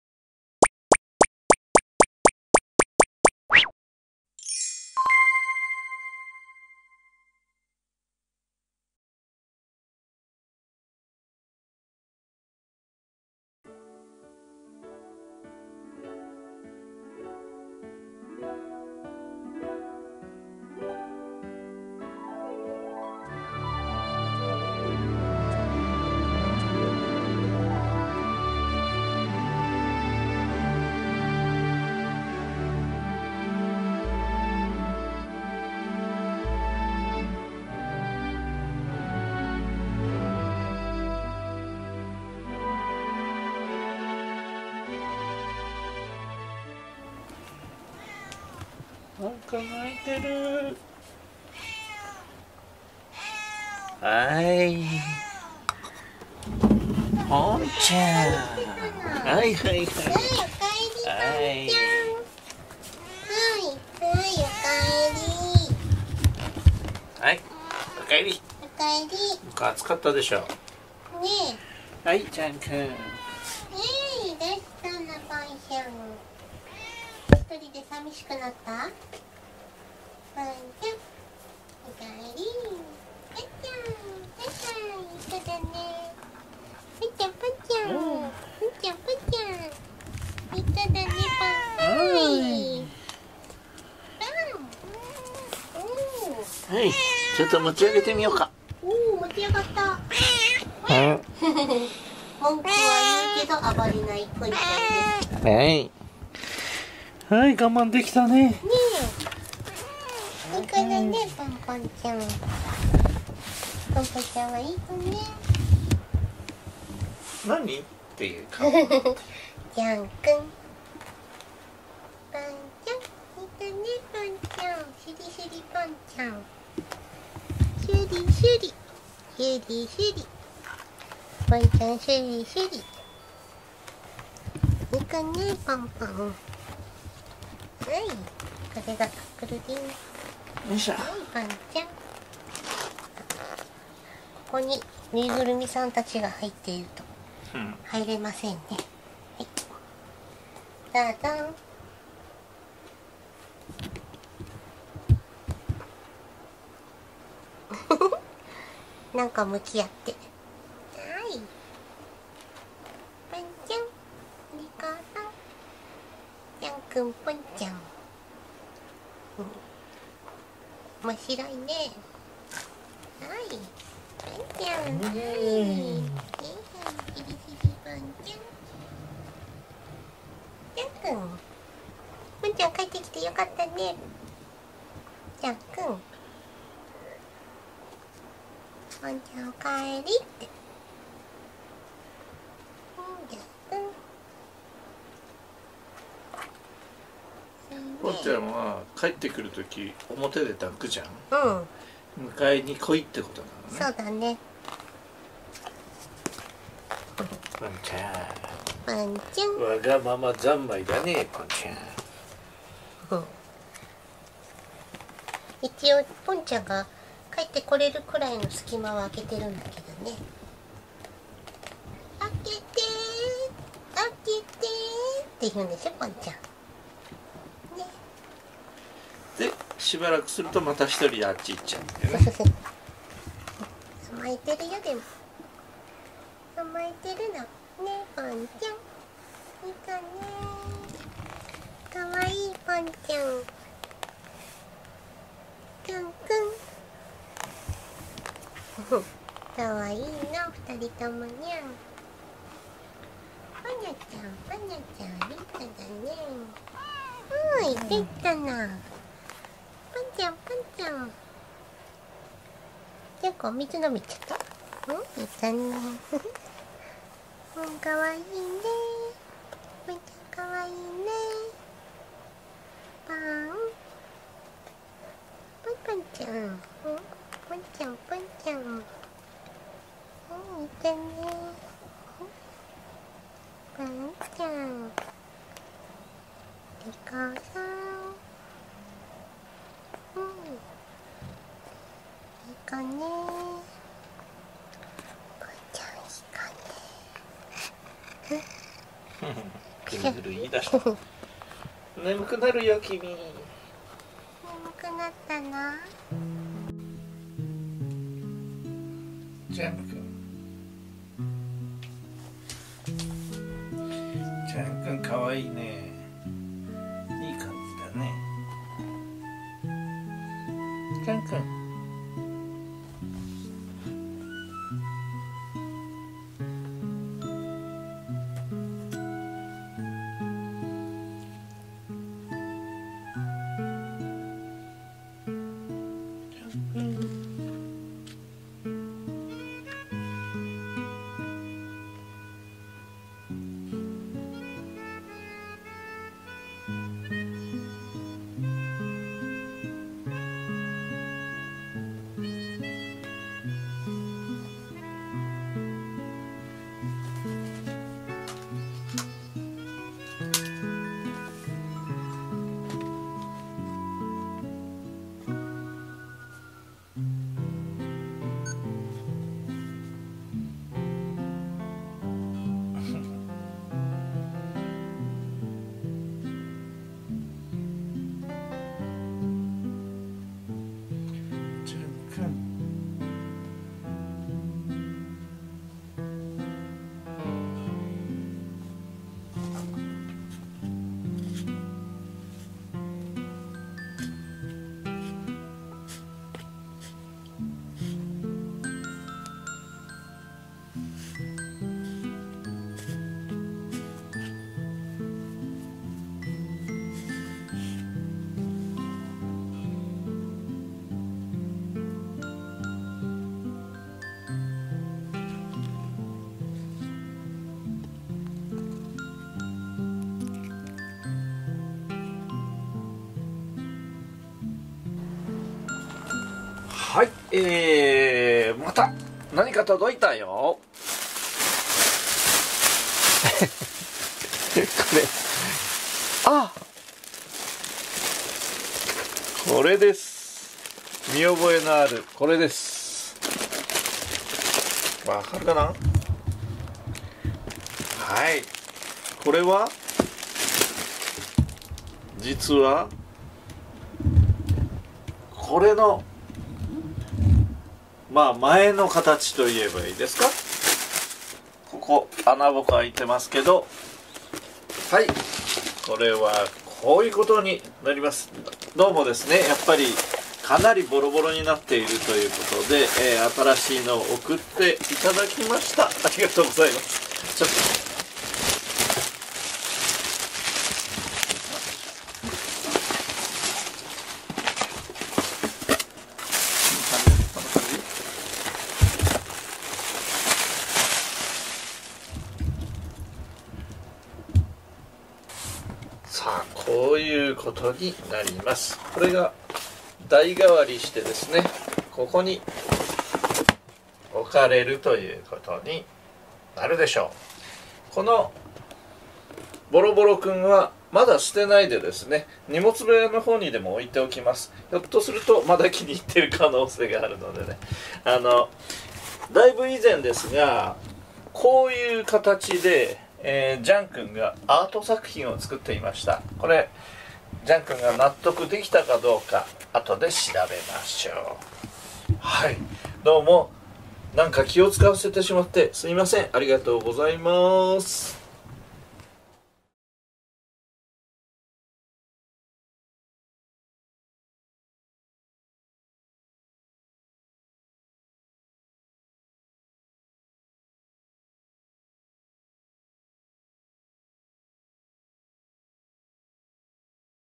パパのキスを文句を言いながらも我慢するポンちゃん【Jean & Pont 2177】2020/7/10
ポンちゃんは人を避けている保護猫でしたが最近になってやっとパパの顔を怖がらなくなってきました。
怖くはないけど迷惑って感じは伝わってきますが、この先のこともあるのでここはちょっと我慢していただいてキスや頬擦りをあえてやっています。
人に甘えることを少しずつ覚えて日々の生活が楽しいと感じられるように。
もうすぐmaoiママのお誕生日です。
お祝いの動画を作ってプレミア配信いたしますので是非ご覧ください。

2020年ジャン＆ポンカレンダー
2020年キトンシッティングカレンダー（ラブ、シオン、アンズ、チャコ、カオリン）